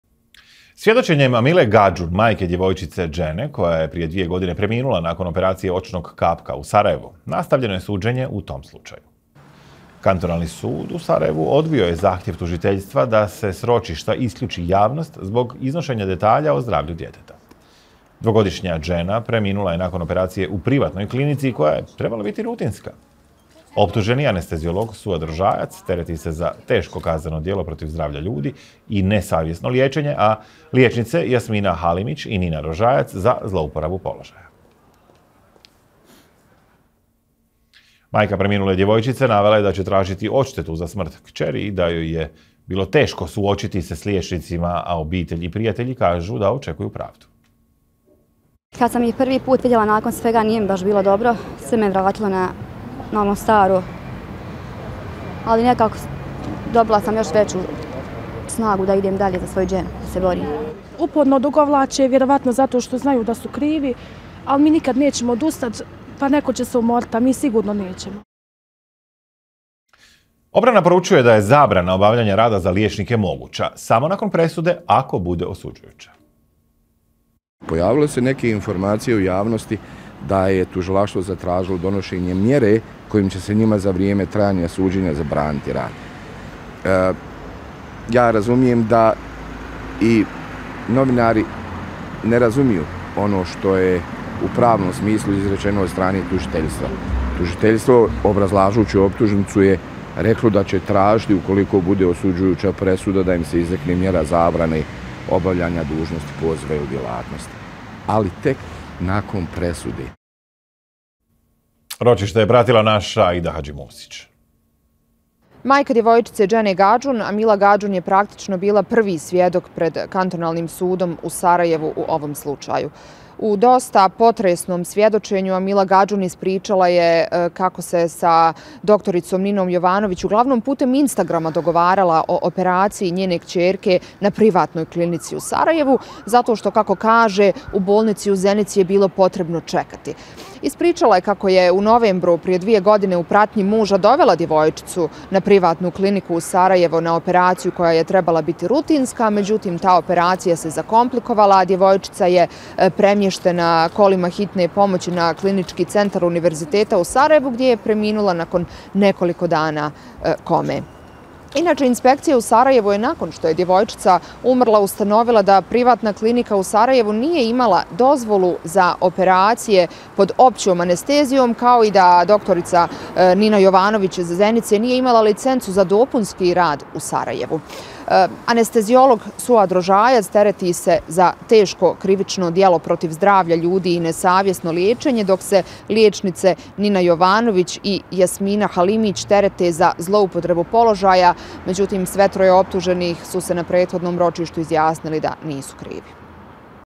Potresno svjedočenje majke djevojčice Džene Gadžun
Svjedočenjem Amile Gadžun, majke djevojčice Džene, koja je prije dvije godine preminula nakon operacije očnog kapka u Sarajevu, nastavljeno je suđenje u tom slučaju.

Kantonalni sud u Sarajevu odbio je zahtjev Tužilaštva da se s ročišta isključi javnost zbog iznošenja detalja o zdravlju djeteta.

Dvogodišnja Džena preminula je nakon operacije u privatnoj klinici koja je trebala biti rutinska.

Optuženi anesteziolog Suad Rožajac se tereti za teško krivično djelo protiv zdravlja ljudi i nesavjesno liječenje, a ljekari Jasmina Halimić i Nina Rožajac za zloupotrebu položaja.

Majka preminule djevojčice navela je da će tražiti odštetu za smrt kćerke i da joj je bilo teško suočiti se s ljekarima, a porodica i prijatelji kažu da očekuju pravdu.

Odbrana poručuje da je zabrana obavljanja rada za ljekare moguća samo nakon presude, ako bude osuđujuća.

Ročište je pratila Aida Hadžimusić.